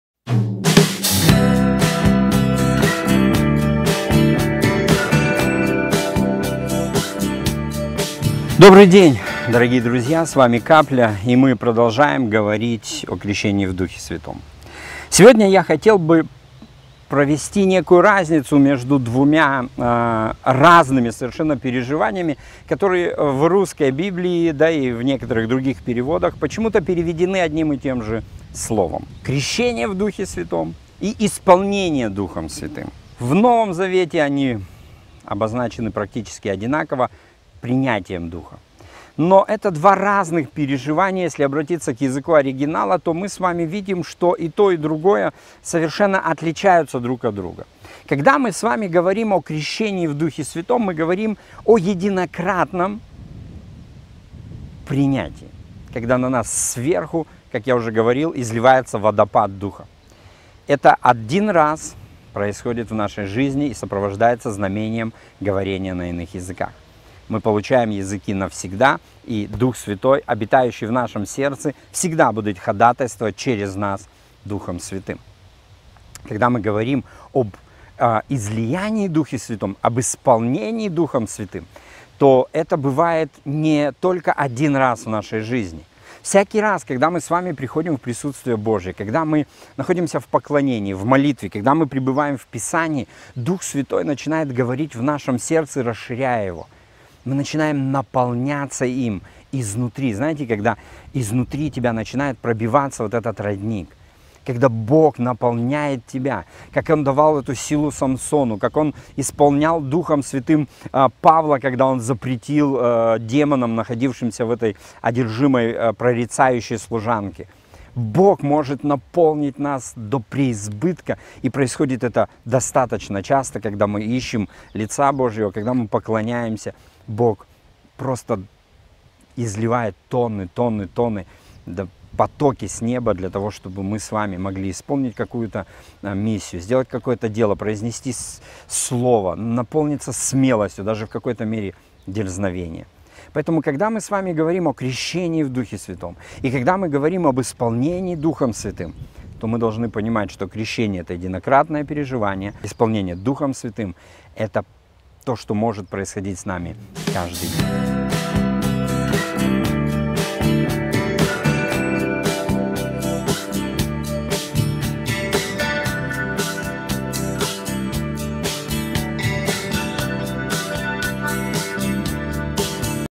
КРЕЩЕНИЕ В ДУХЕ И ИСПОЛНЕНИЕ ДУХОМ
Программа «Капля». Крещение в Духе Святом. Кто крестит Духом? Когда и как это происходит? Иные языки и существуют они сегодня? Мы постараемся подробно обсудить многие вопросы, связанные с крещением в Духе Святом. Крещение в Духе Святом и исполнение Духом, это разные переживания.